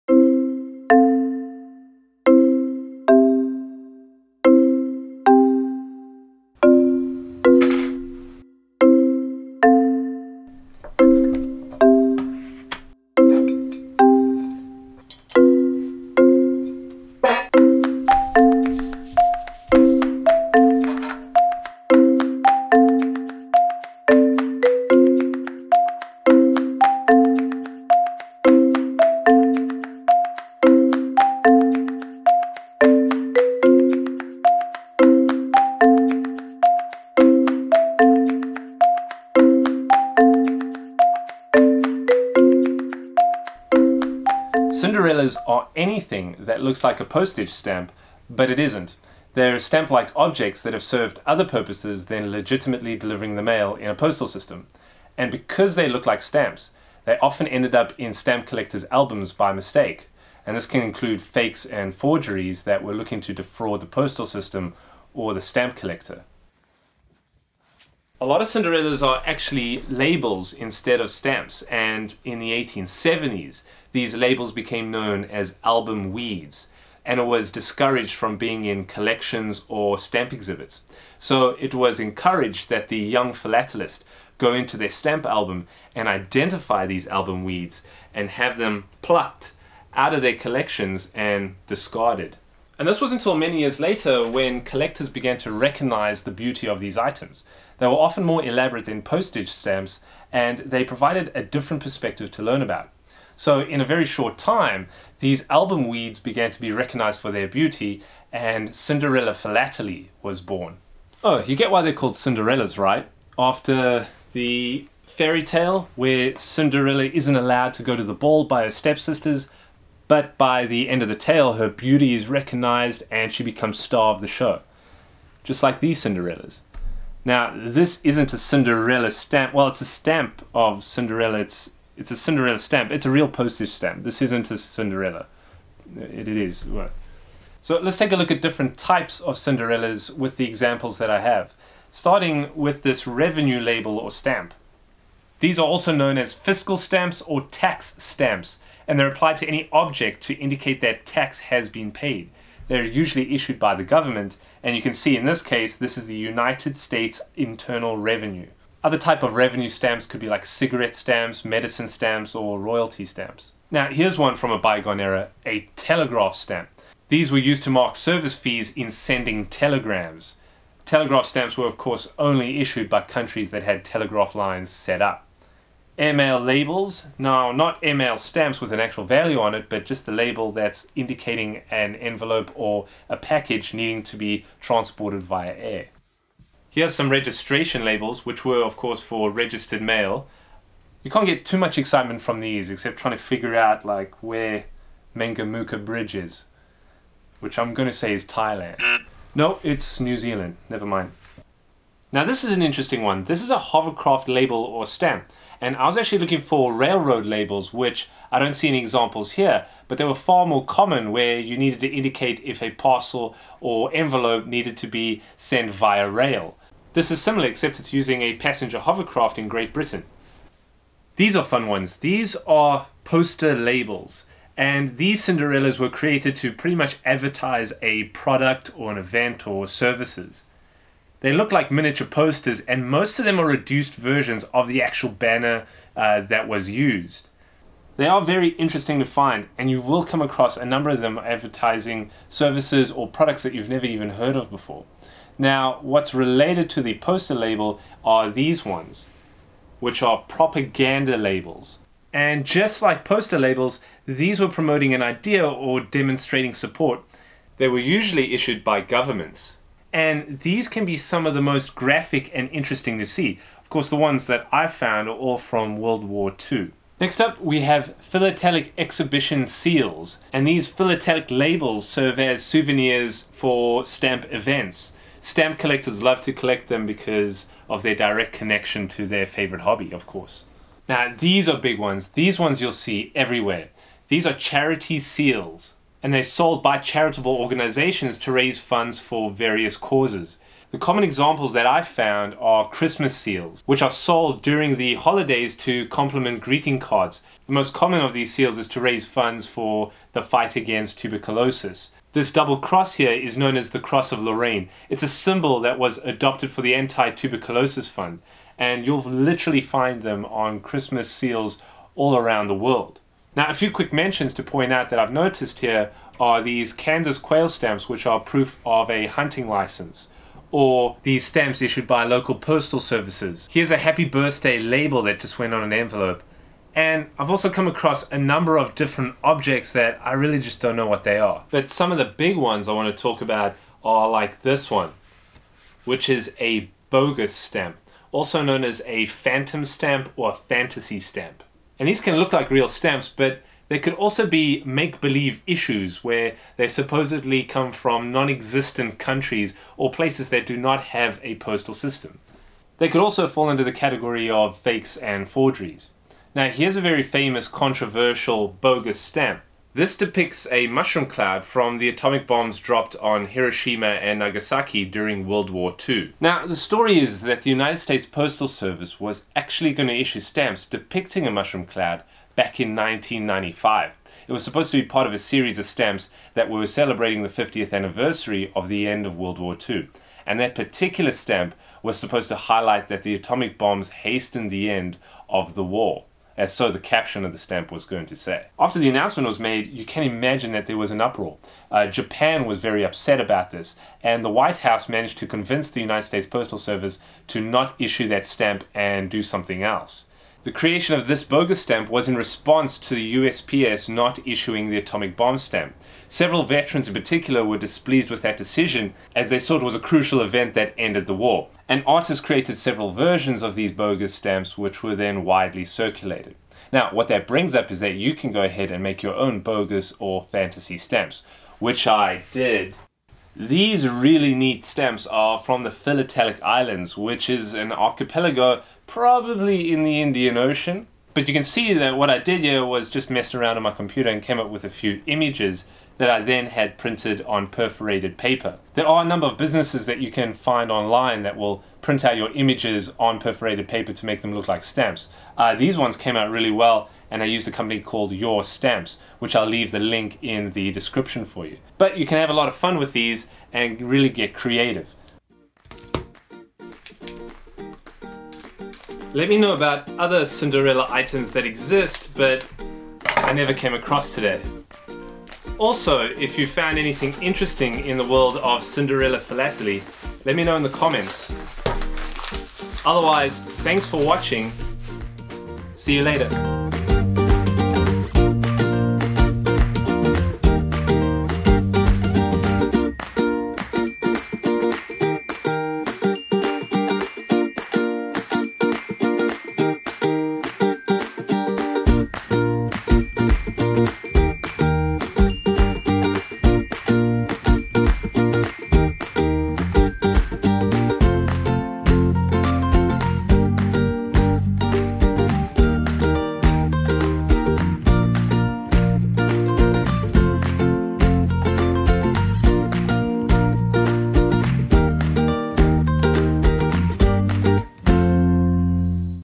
What are Cinderella Labels?
Anything that looks like a postage stamp, but isn't.  Here we take a look at the concept of Cinderellas in philately and the various types that you can find, including revenue stamps, registration labels, poster and propaganda labels, and more.